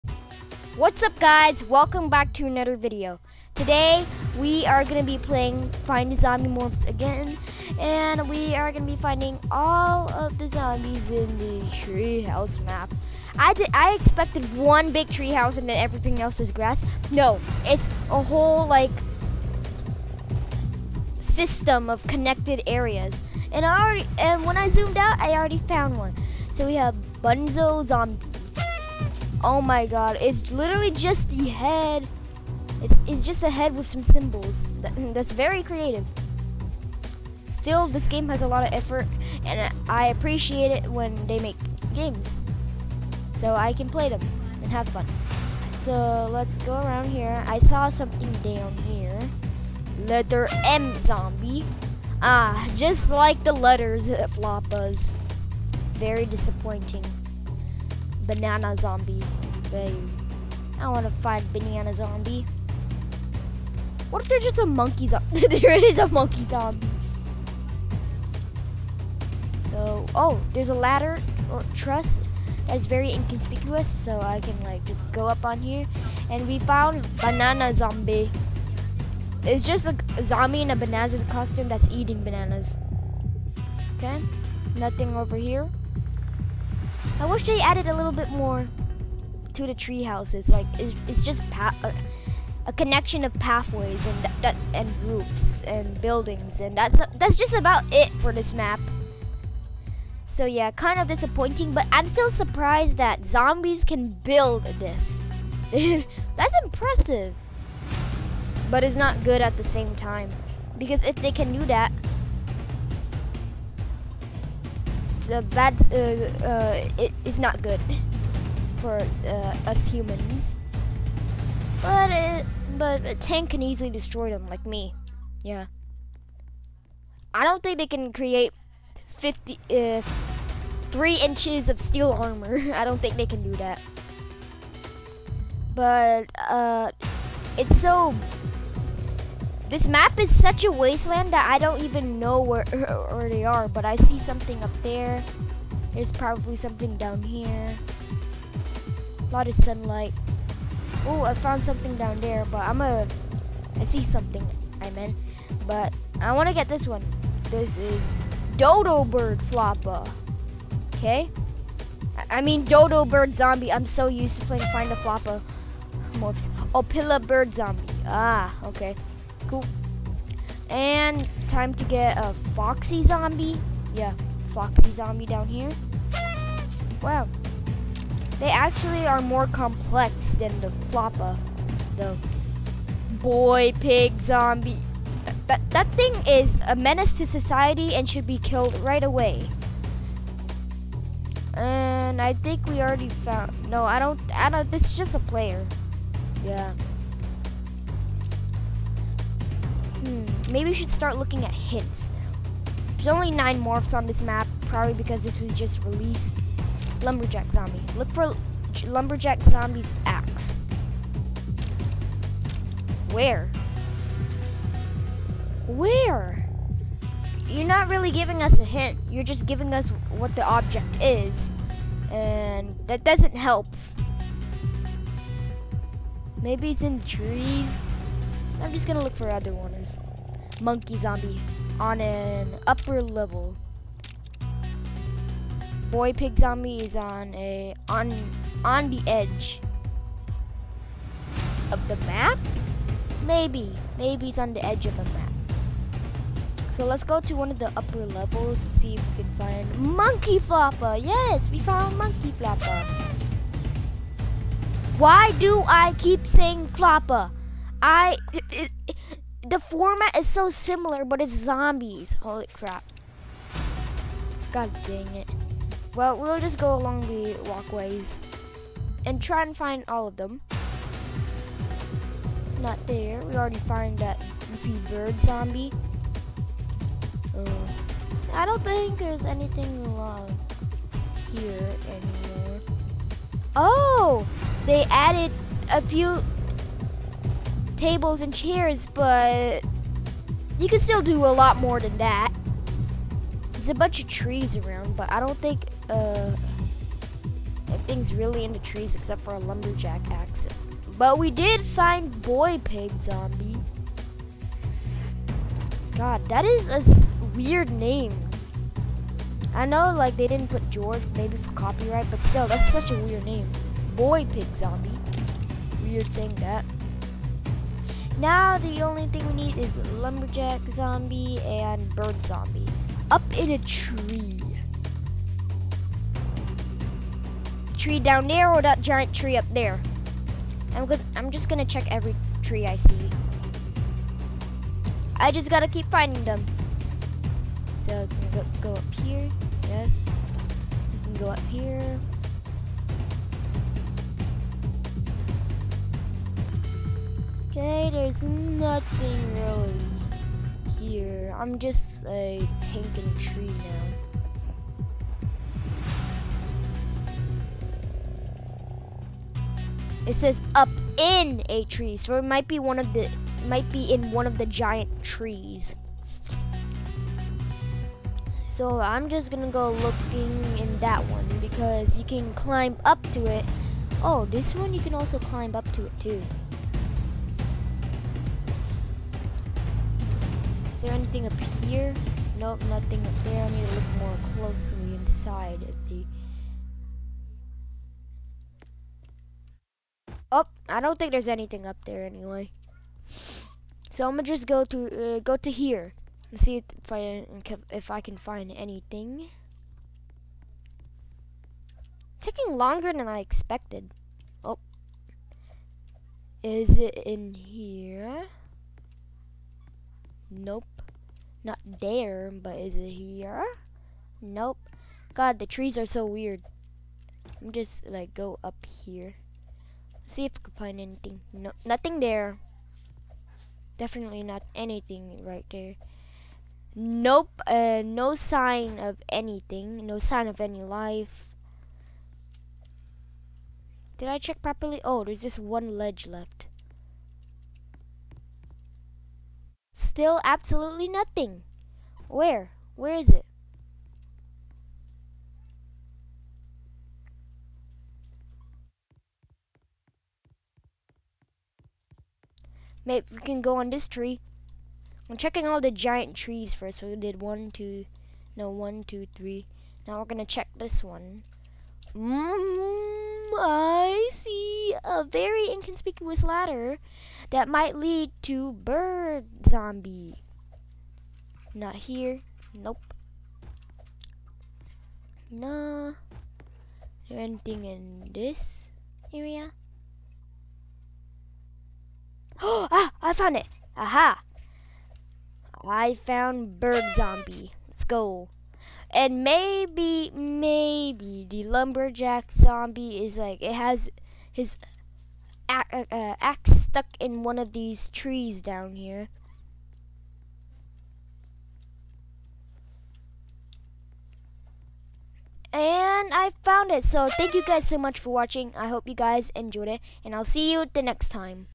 HOW DO ZOMBIES MAKE TREEHOUSES?
I found all of the zombies in the treehouse map.